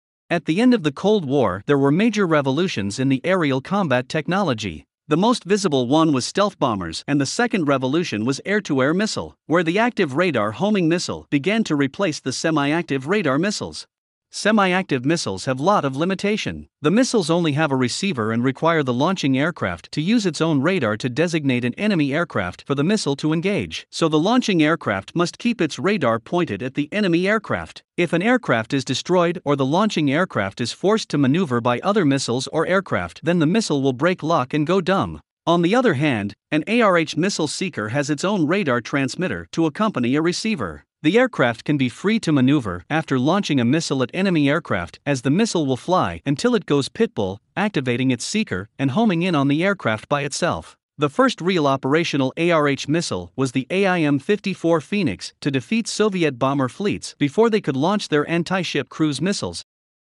How "Smart" Missiles Became So Intelligent
At the end of the Cold War, The active radar homing (ARH) air-to-air missile began to replace semi-active radar homing (SARH) air-to-air missiles as the primary medium and long-range missiles that armed every fighter.An active radar homing missile differs from a semi-active radar homing missile in that the missile’s seeker has its own radar transmitter to accompany a receiver. Semi-active missiles only have a receiver and require the launching aircraft to use its own radar to “designate” an enemy aircraft for the missile to engage

𝐅𝐨𝐨𝐭𝐚𝐠𝐞 𝐂𝐫𝐞𝐝𝐢𝐭𝐬:
Footage courtesy of public domain archives of U.S Department Of Defense (DoD)
Special thanks to US Navy for maintaining public domain archives of military footage.